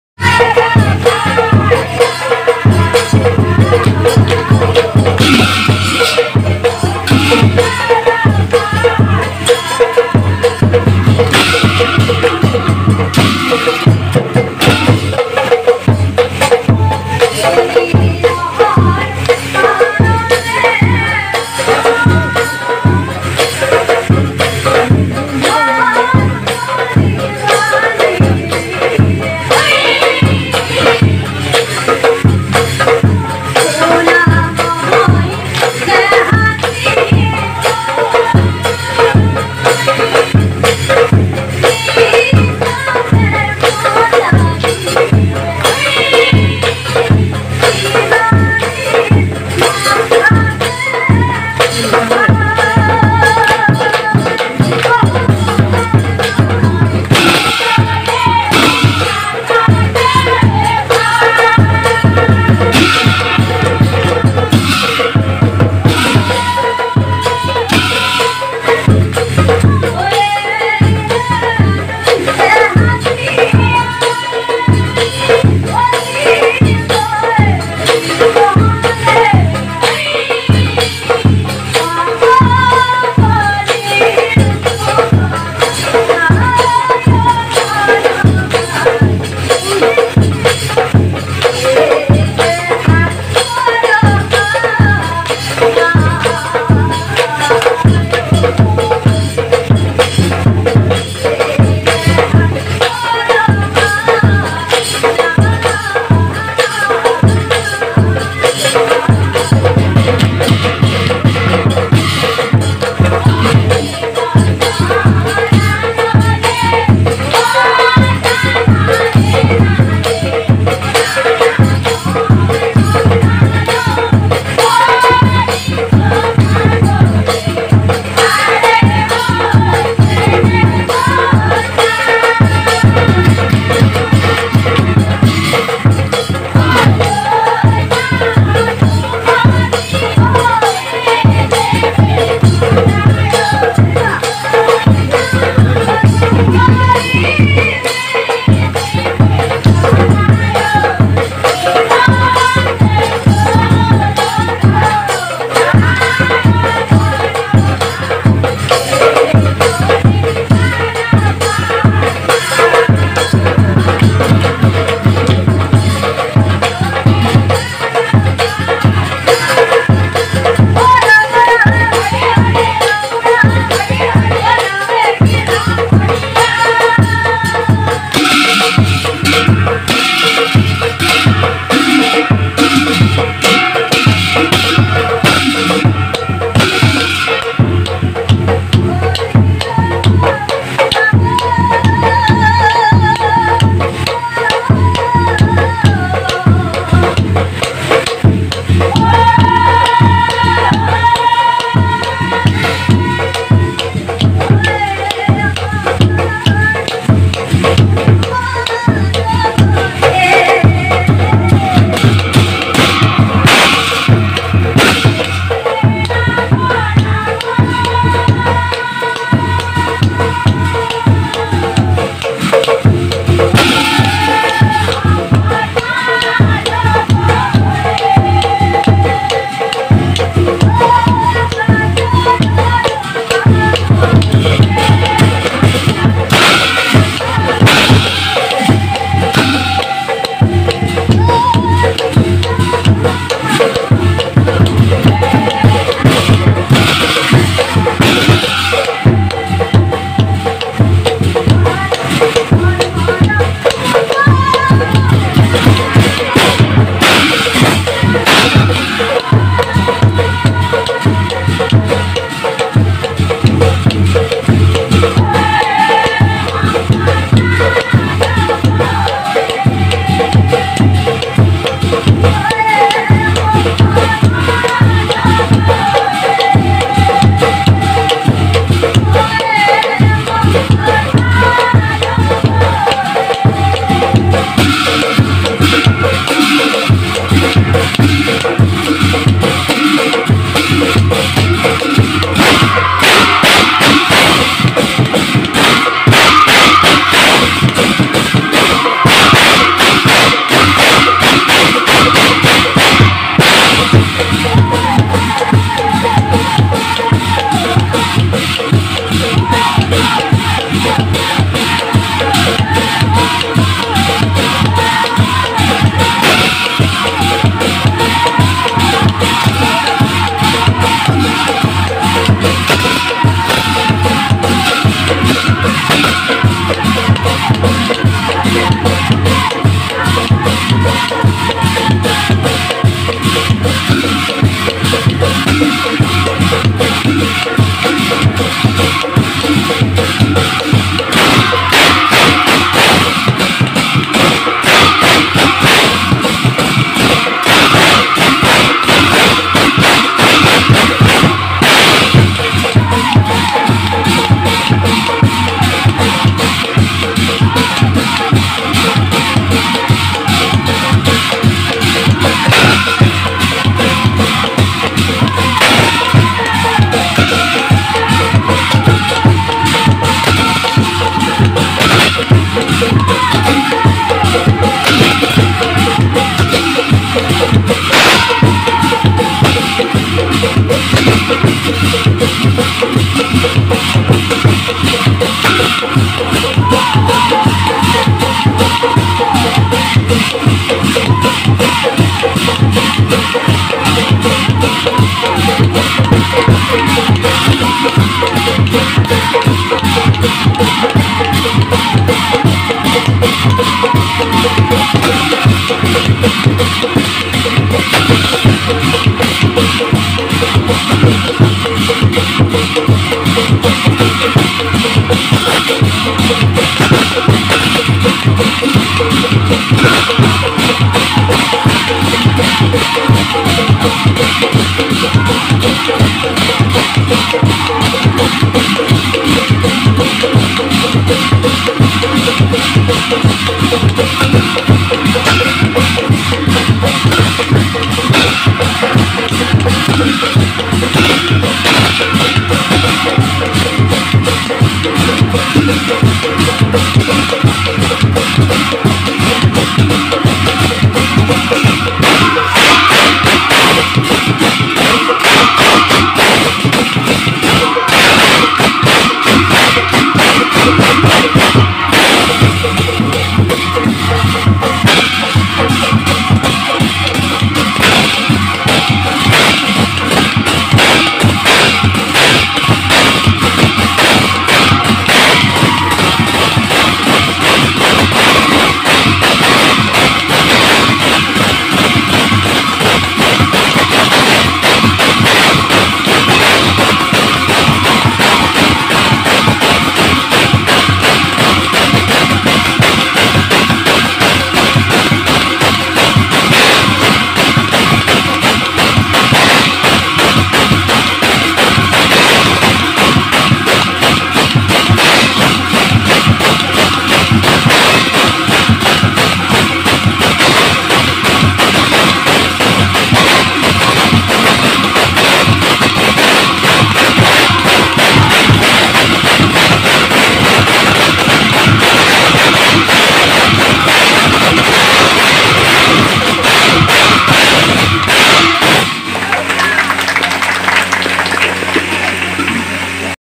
Mitali Baruah ৷৷ nagara naam ৷৷ এইবাৰ ভোৰতালেৰে চমক দিলে৷ কোন বাৰু?
পুনৰ এগৰাকী নতুন প্ৰতিভাসম্পন্ন মহিলা পাঠক

Hi friends
I am Dipak kumar from assam .
Nagara naam is a folk song of assam.
I like to write and sing this song.
Today I published this video .
Nagara naam new artist Mitali Baruah
Recording place : Dhulara

🎈আপোনাৰ ভিডিঅ’ প্ৰচাৰ কৰিবলৈ
 তলৰ নম্বৰত যোগাযোগ কৰক👇

মোক
Instagram ত follow কৰিব পাৰে👇❤️